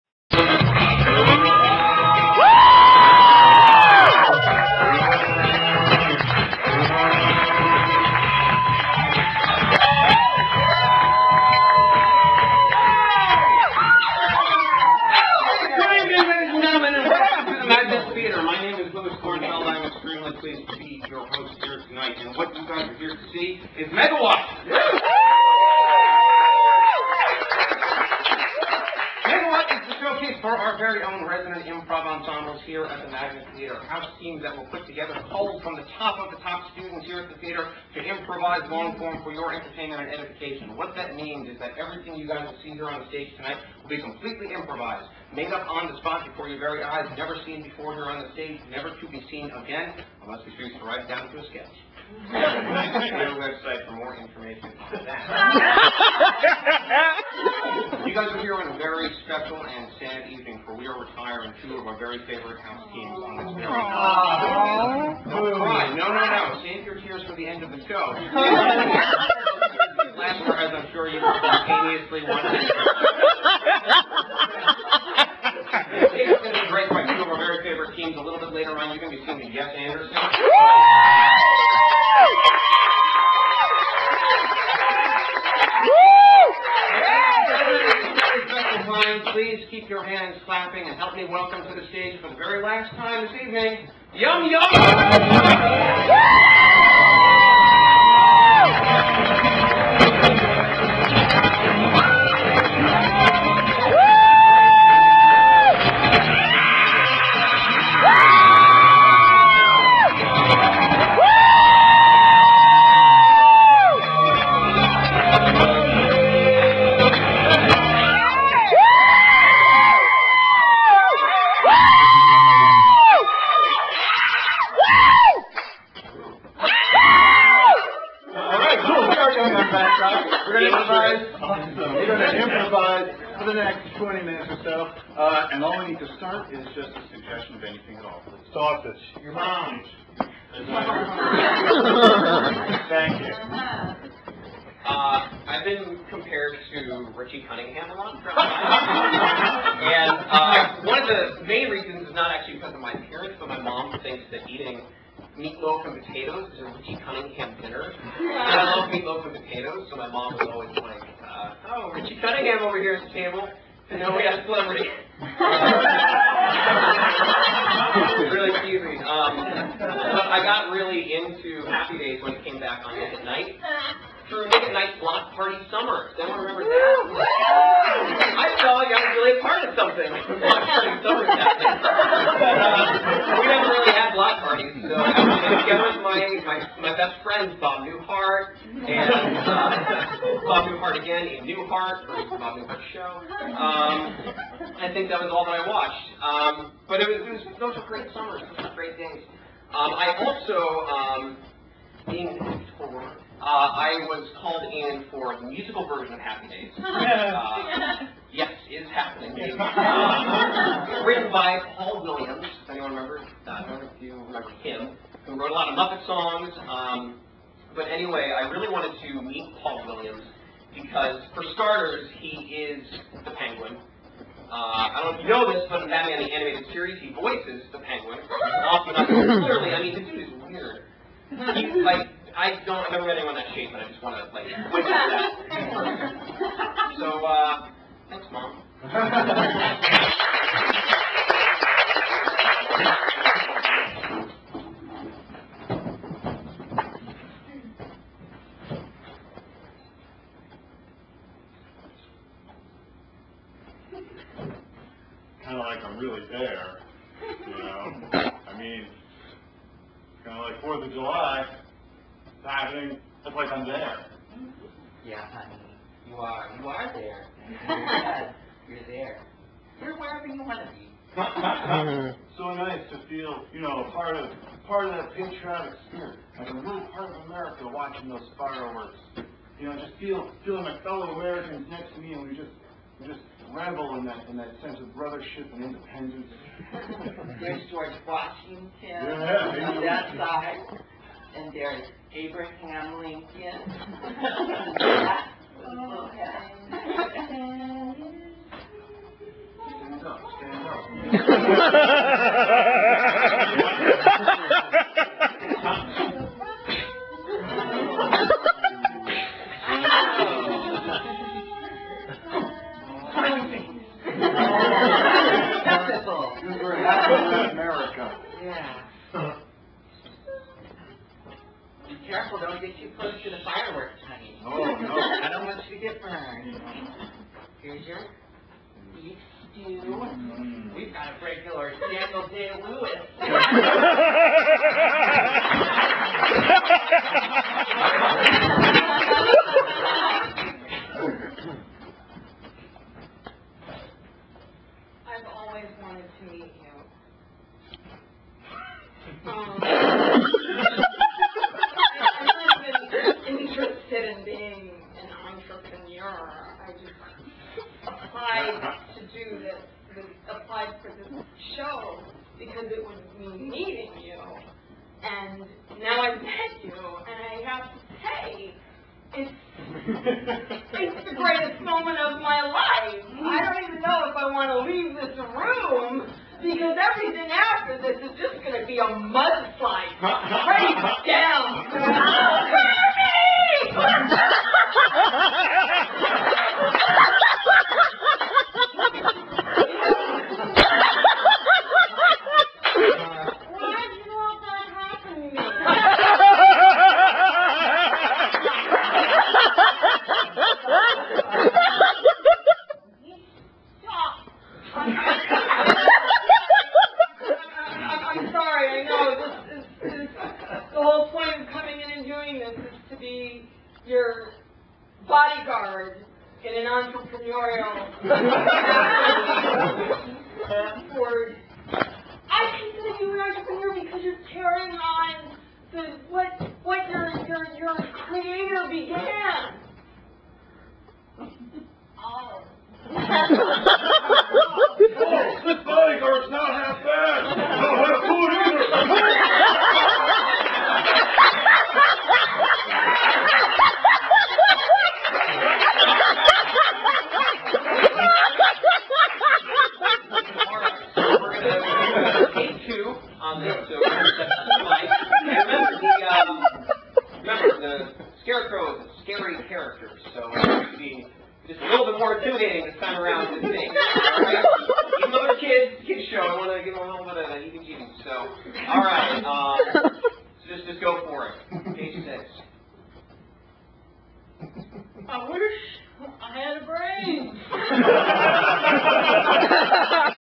The Last Yum Yum Bangkok show, Part One
It's a few months old, but finally, here's the last ever yum Yum Bangkok show. YYB was a house team at the Magnet Theater featuring Leanne Linsky, Rob Morrison, Mary Regan, Jess Lane, Matt Koff, Mike Yoder, and me, Michael Short. I'm the tall guy who gets the suggestion at the beginning.